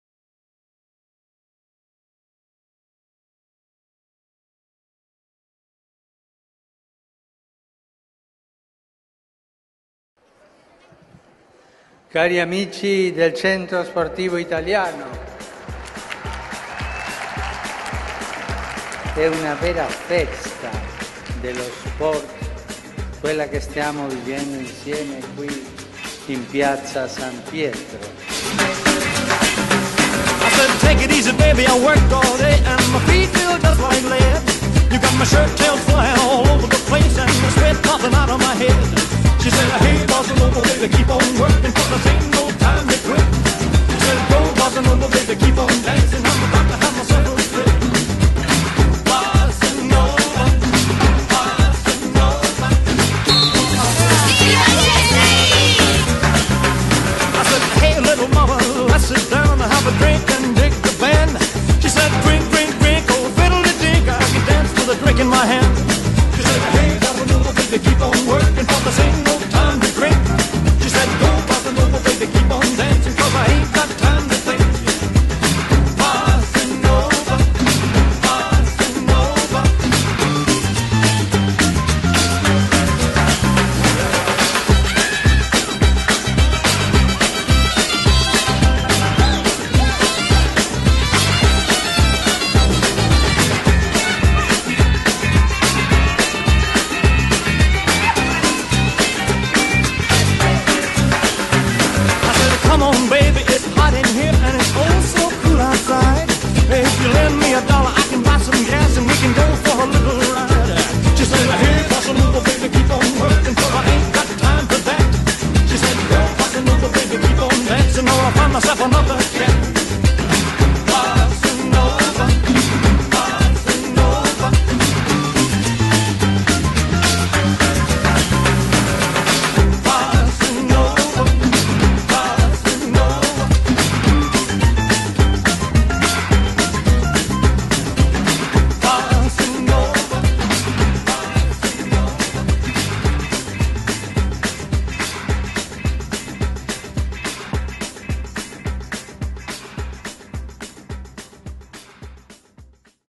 Clip 7 giugno 2014 | CSI incontra Papa Francesco
Sabato 7 giugno 80mila giovani atleti, con i loro allenatori, dirigenti e famiglie sono arrivati a Roma per festeggiare con Papa Papa Francesco i 70 anni di fondazione del Centro Sportivo Italiano.